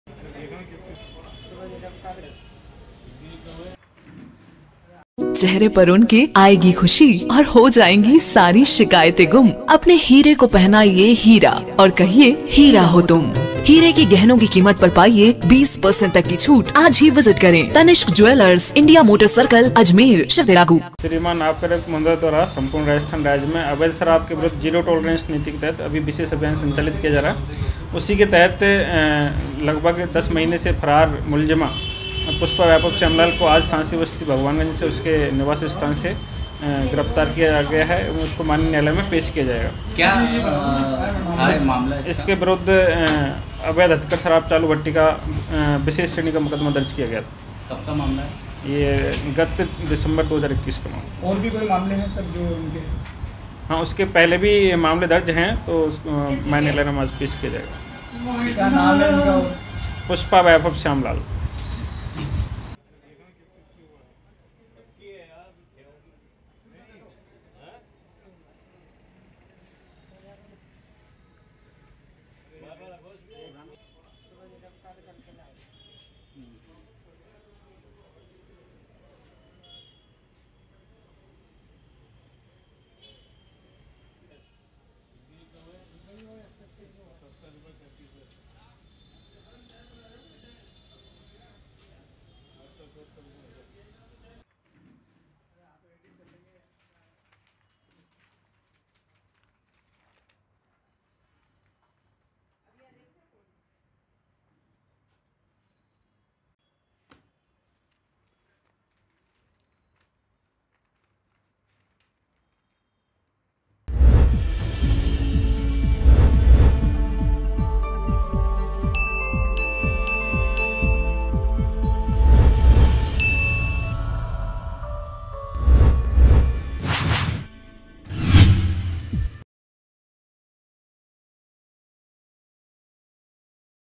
Ajmer | 10 महीने से फरार चल रही आरोपी पुष्पा को आबकारी पुलिस ने किया गिरफ्तार | MTTV INDIA
10 महीने से फरार चल रही आरोपी पुष्पा को आबकारी पुलिस ने किया गिरफ्तार
अवैध हथकड़ शराब और चालू भट्टी चलाने का था मामला
सांसी बस्ती भगवान गंज से आरोपी को किया गिरफ्तार